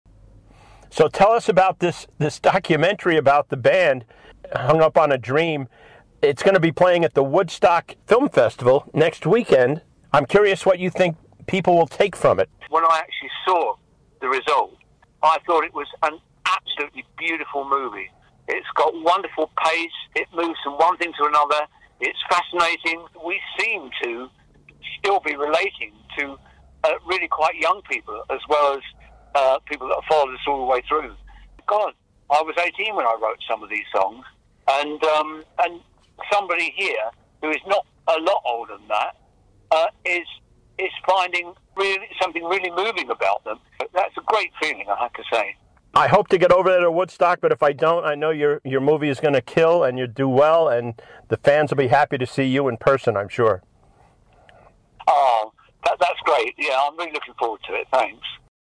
Meet Rod Argent, band member of The Zombies in an interview with Mike Bennett from Radio WHUD ☮️
“But this is the other extraordinary thing that, we seem to still be relating to really quite young people as well as people that have followed us all the way through.”

THIS FRIDAY! @ 8PM @ Bearsville Theater

HUNG UP ON A DREAM: “THE ZOMBIES DOCUMENTARY” | Robert Schwartzman | U.S Premiere☮️

Followed by a special live acoustic performance from Rod Argent and Colin Blunstone of The Zombies

When ranking the iconic British invasion’s most seminal bands like the Beatles, Rolling Stones, The Who, and the Kinks, the Zombies are often overlooked. However, filmmaker Robert Schwartzman rectifies this with "Hung Up on a Dream: The Zombies Documentary," shedding light on the charming group, including Rod Argent, Colin Blunstone, Chris White, Hugh Grundy, and the late Paul Atkinson. 🇬🇧